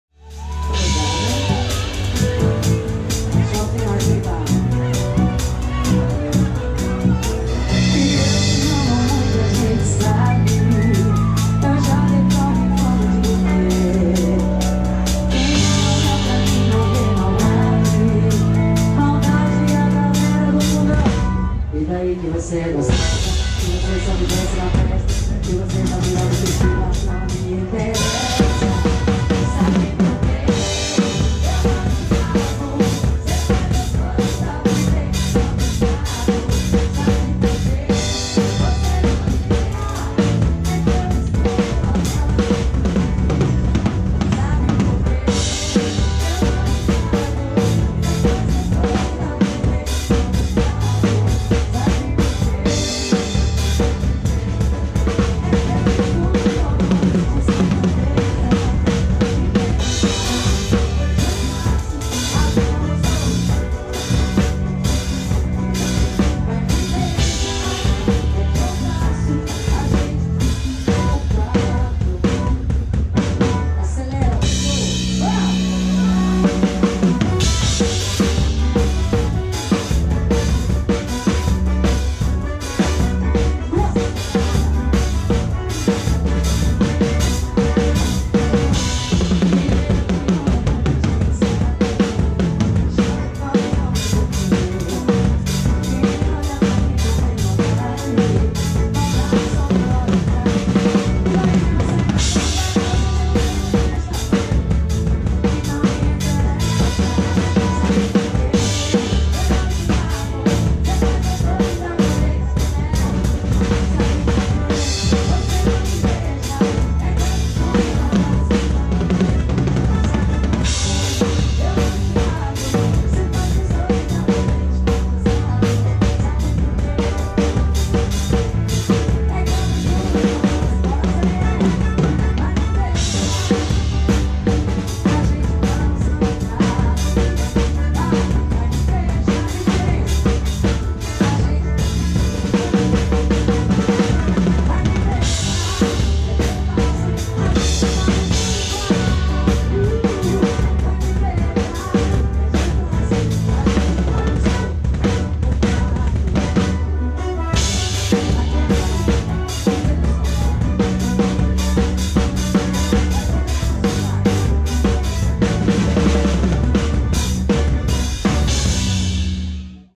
Último show no Brasil (Pierre Maskaro) Férias em Portugal
Fala Galerinha, esse foi minha última participação no Brasil e agora vou passar umas férias em Portugal